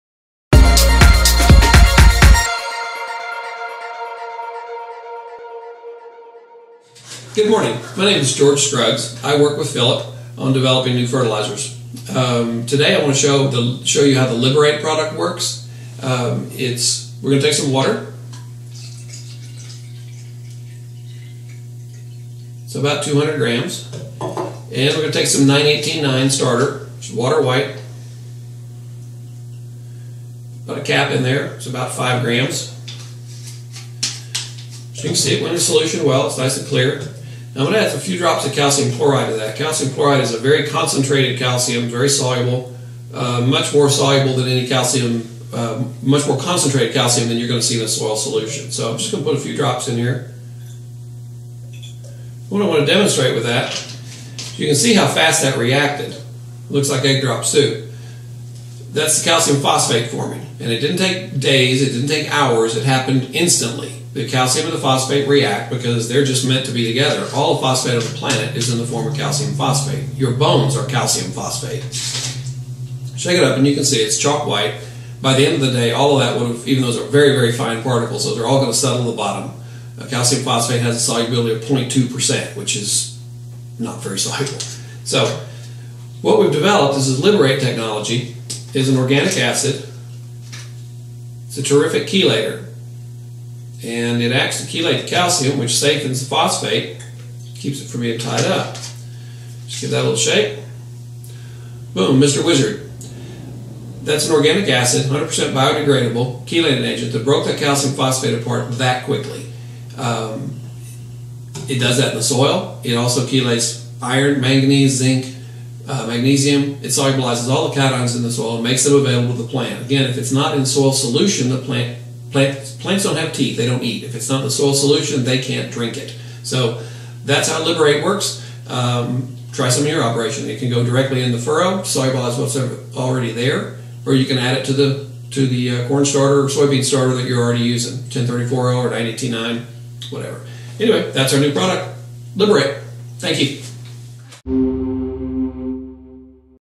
Maximize Your Phosphorus Investment - With Liberator by Genesis Ag
Maximize the Phosphorus Uptake in Your Plants.  Genesis Ag's Liberator will release the phosphorus, calcium, potassium, magnesium, and beneficial micronutrients in your soils for uptake into your plants. Stop wasting money pouring phosphorus into your soils, only to have them "eaten up" by the calcium and other cations in your soil.  Stop feeding the insatiable soil complex and Start feeding your plants! :-)